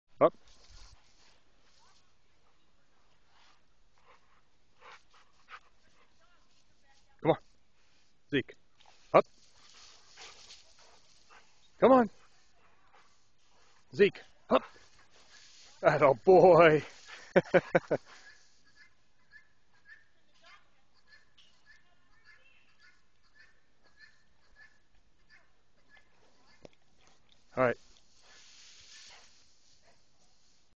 Zeke (Bouvier) Jumps on Hay Bale
Zeke  loves to jump atop stuff.